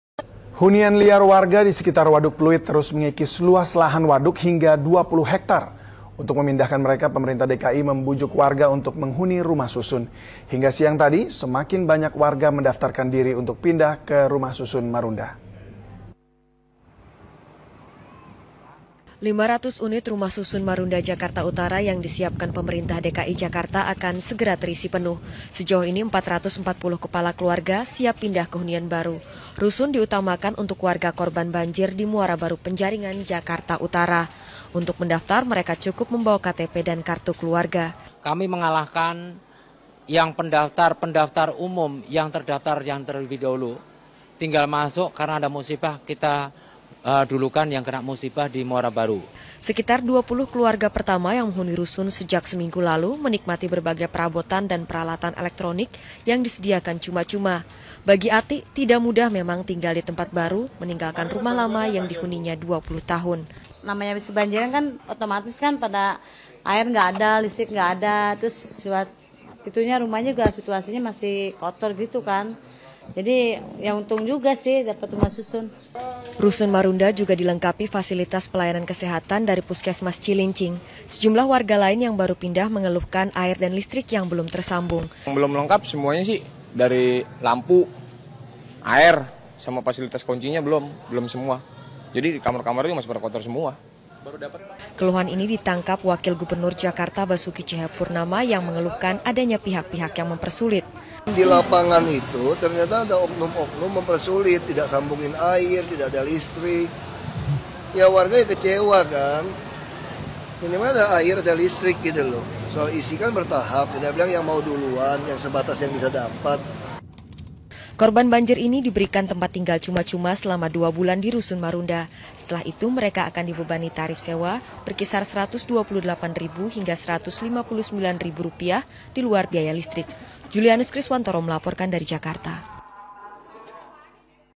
Oknum Pemda DKI Penyebab Rusun Marunda Kekurangan Fasilitas
Jasa Youtube Views, Indonesia Facebook Fans dan Twitter Follower.

Ahok mengakui adanya oknum Pemda DKI yang menjadi penyebab kurangnya banyak fasilitas di rusun Marunda.

Pak Ahok dan Jokowi sudah mengusahakan yang terbaik, ayo dong buat para oknum Pemda DKI atau masyarakat, jangan selalu memikirkan keuntungan sendiri tapi buat perbaikan semua. Jangan ada lagi oknum yhang suka memeras.